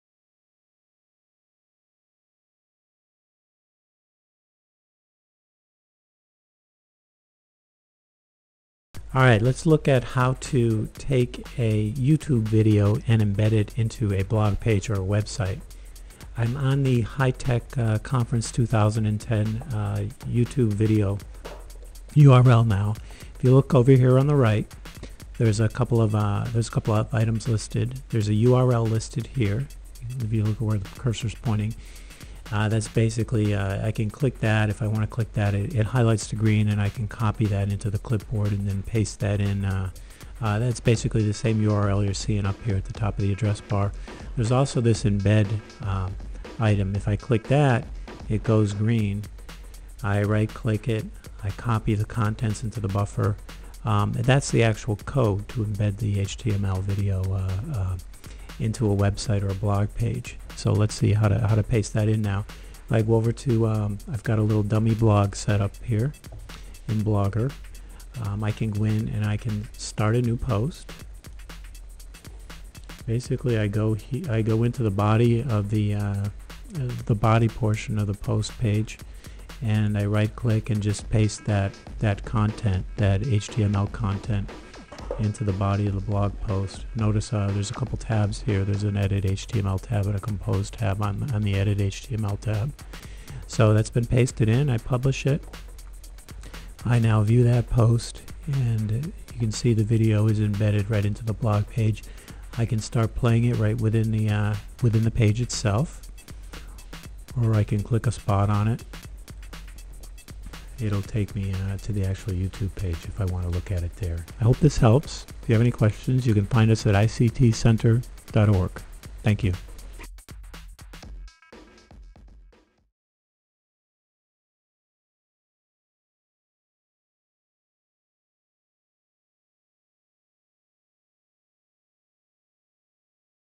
How to Embed a YouTube Video
In this video tutorial Gordon Snyder demonstrates how to embed a YouTube video into your blog or web page.